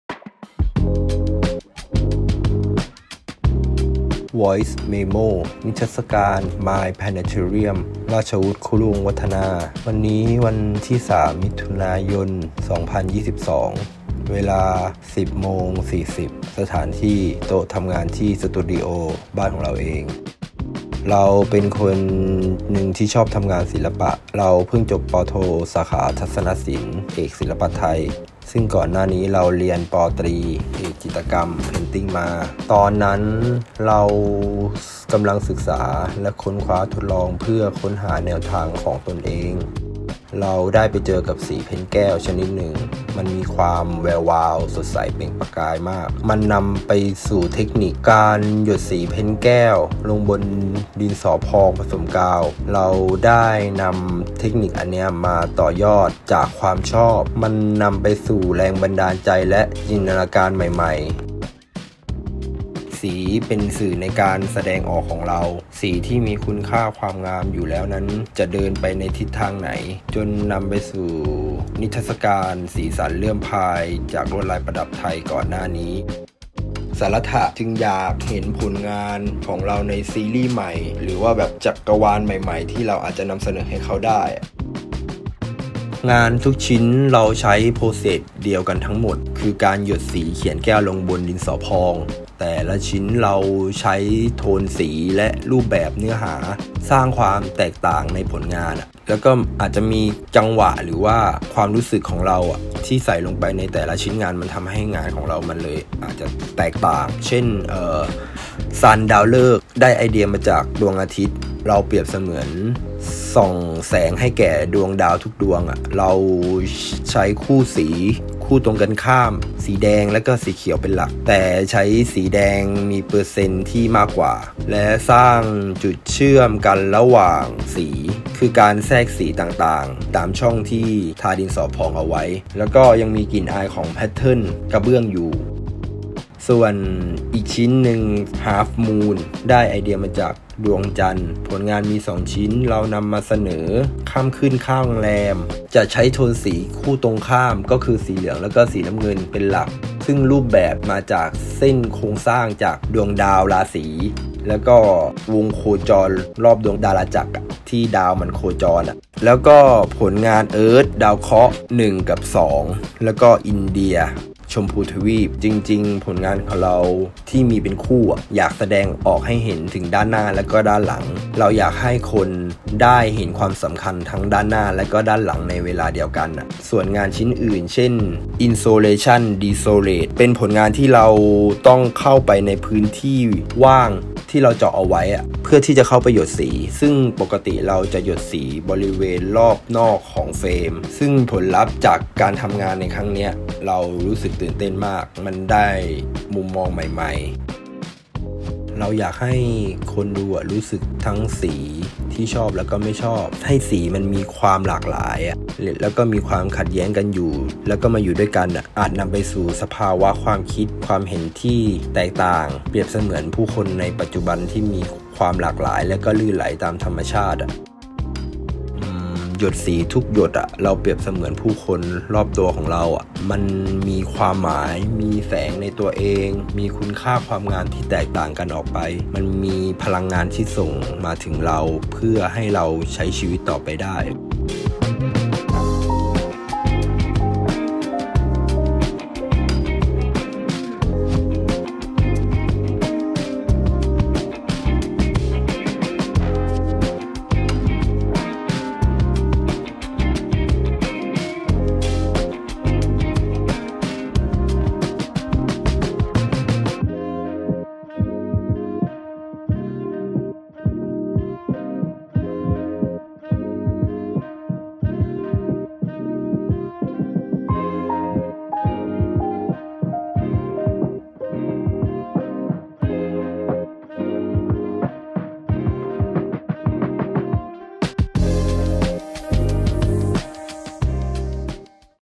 VOICE MEMO | Ratchawoot - Mind Planetarium Exhibition
Saratta Artist Voice Memo — Archiving artist thoughts and work in process narrated by artist voice memo.

Saratta presents
'Mind Planetarium จักรวาลของจิตใจ'
Flowing mind through fluid colors exploration
by Ratchawoot Kuruwongwattana
14 May - 19 June 2565

‘จักรวาลของจิตใจ’ โดย ราชวุธ คุรุวงศ์วัฒนา เป็นการชวนสำรวจคุณค่าความงามของความอิสระตามธรรมชาติที่เกิดขึ้นโดยฉับพลันจากกระบวนการทำงาน ณ ขณะนั้น ผ่านมุมมองเรื่องคติภูมิจักรวาลกับการตั้งคำถามเกี่ยวกับการเปลี่ยนแปลงอันรวดเร็วของโลกสภาวะในยุคปัจจุบัน นำเสนอผ่านเทคนิคสีเลื่อมพราย เทคนิคเฉพาะตัวที่เกิดจากการคิดค้นทดลองกระบวนการสร้างงานจิตรกรรมไทย การใช้สีเป็นปัจจัยในการเฝ้าสังเกตุ การเกิดขึ้น ตั้งอยู่ และดับไป ความเปลี่ยนแปลงและผลลัพธ์อันคาดไม่ถึง นำมาซึ่งสาระอันสำคัญที่ถ่ายทอดผ่านผลงานชุดนี้

‘Mind Planetarium’ by Ratchawoot Kuruwongwattana is the invitation to explore the beauty value of immediate freedom in nature. The crafting process through the cosmology concept, in itself, is to inquire about the high-speed existence of the contemporary world. Present by glass paint solvent based on white clay filler. Which is this unique technique that results from the experiment with the process of Thai painting. In particular, works as a lens to observe three marks of existence. The change and unexpected outcomes of the artworks are used to portray this specific subject.

Special Thanks
Media Production by Samsee Production
Catering from Reno All-day Dining Reno Hotel Bangkok
Reggae Music Live session by Malaiman Downtown